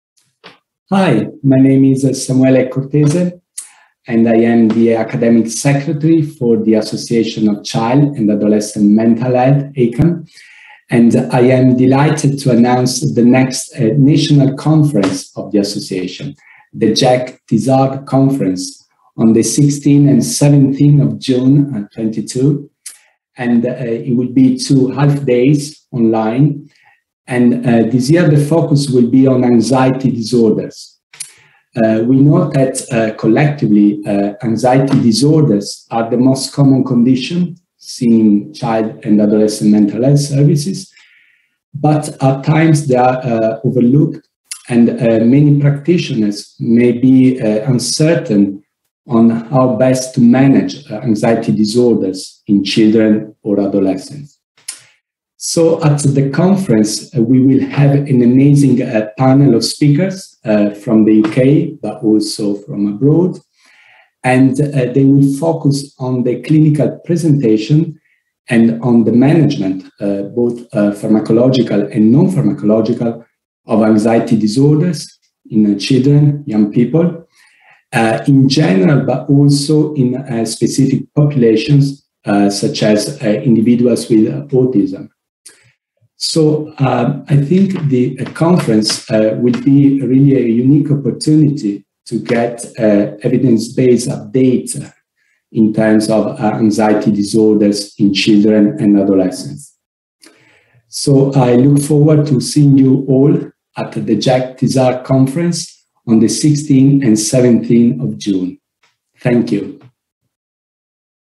Updates on Anxiety in children and adolescents – Jack Tizard International Conference
The 2022 ACAMH Jack Tizard International Conference will provide an updated and evidence-based overview on the clinical presentation and management of anxiety disorders in the clinical setting as well as in schools.